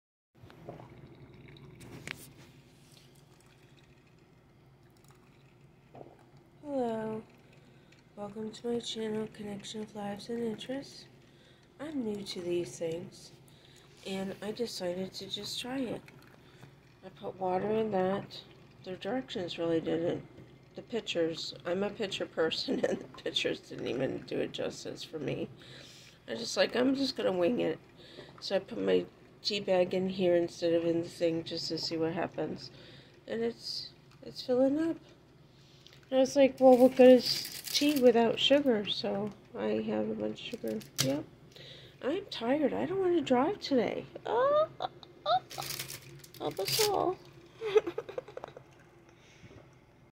for tea coffee tired 😜😜😜(Not for kids)
Sharon Traveling from Massachusetts to South Carolina Saturday and Sunday June 11th and 12th 2022